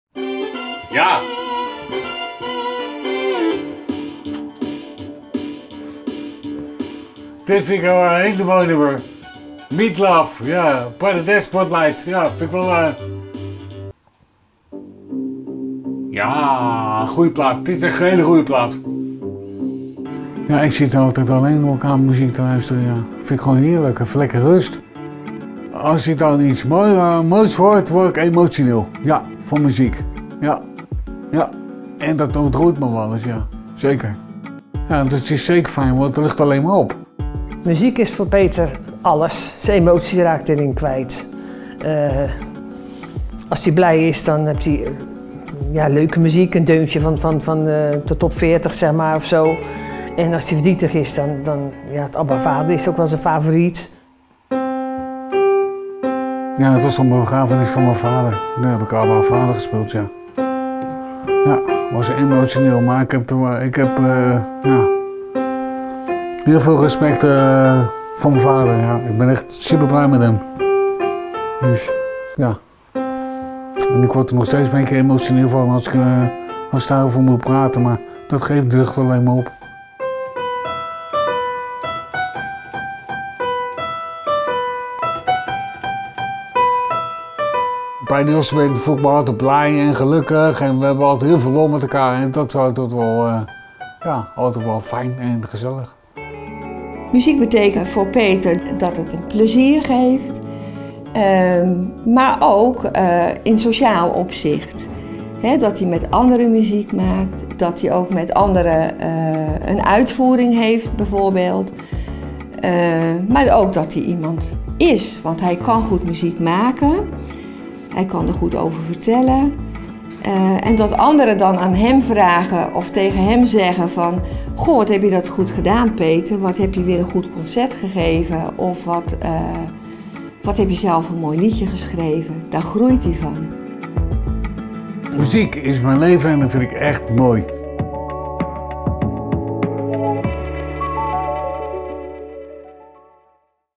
Wat betekent muziek voor Peter?
Peter vertelt wat muziek voor hem betekent